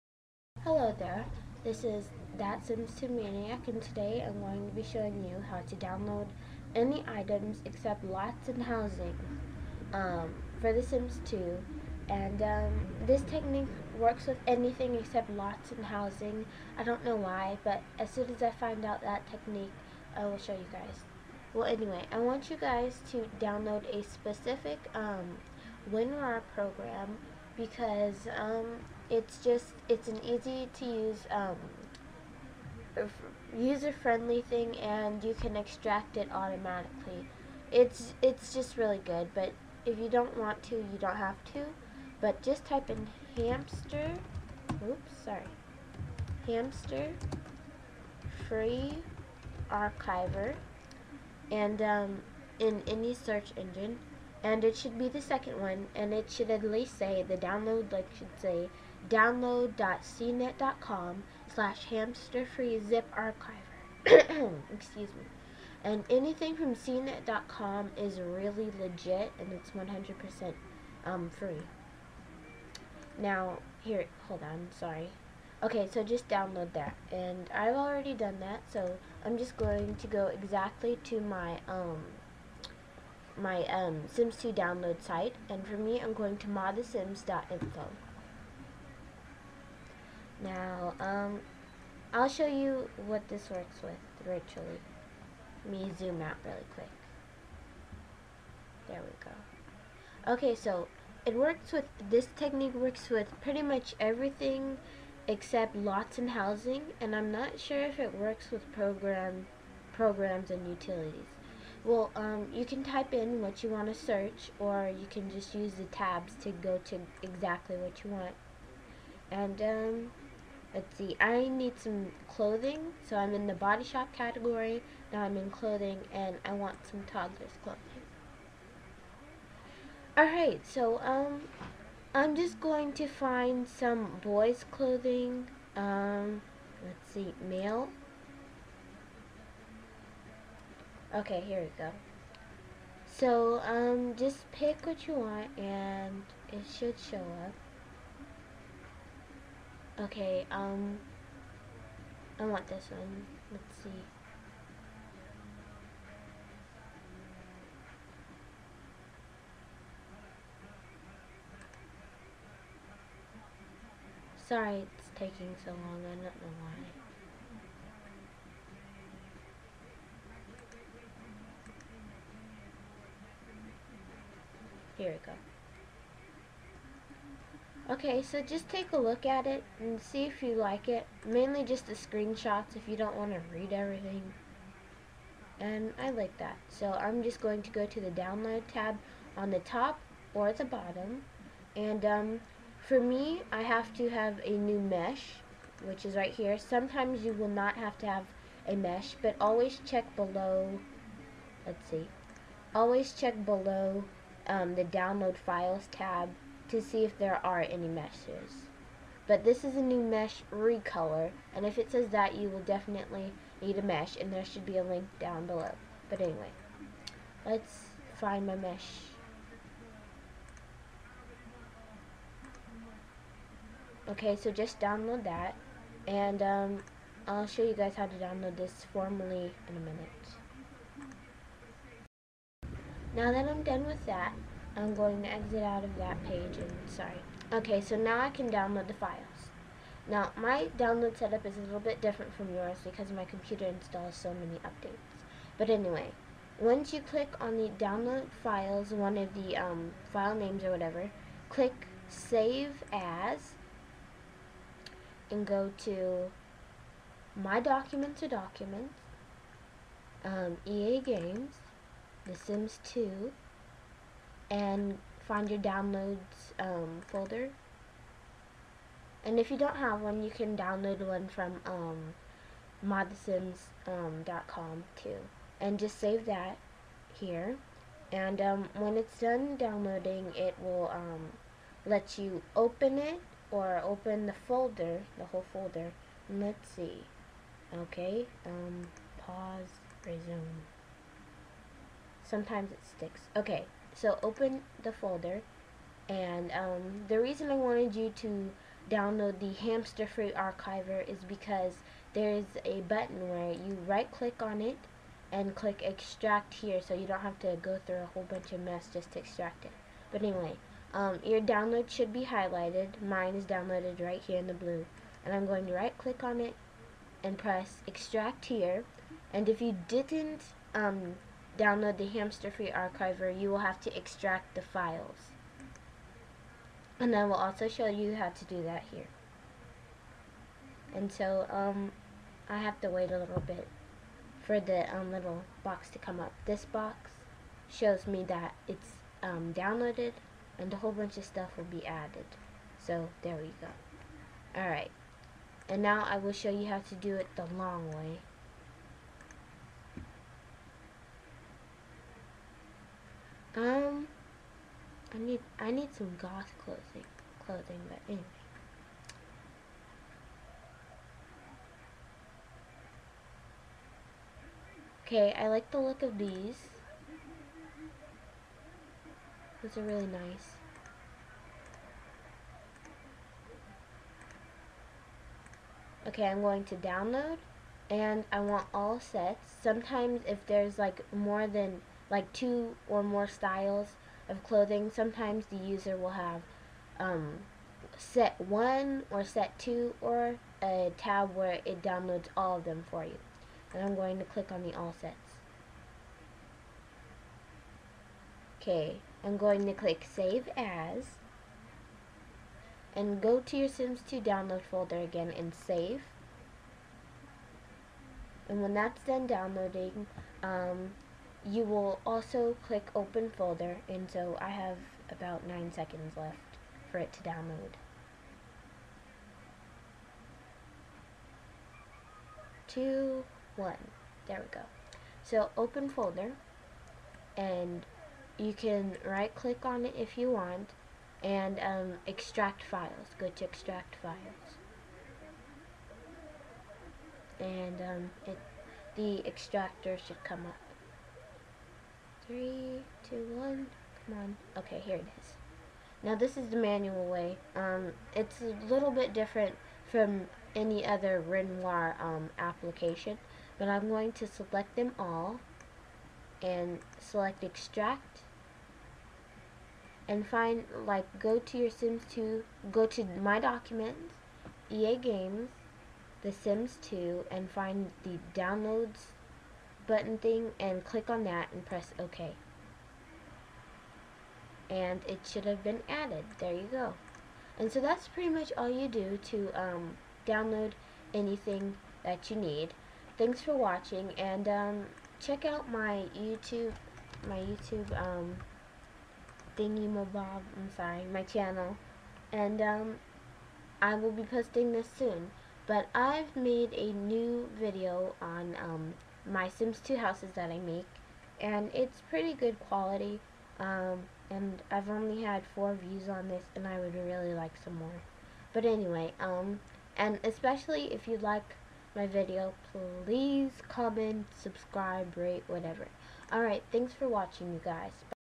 How to download/install Sims 2 Custom Content
YOU HAVE TO HAVE A WINrar PROGRAM INSTALLED ON YOUR COMPUTER!
My way of downloading any items I feel I need. This technique is especially easy for those Sims 2 movie makers who want a quick and easy way to download custom content without having to do it manually.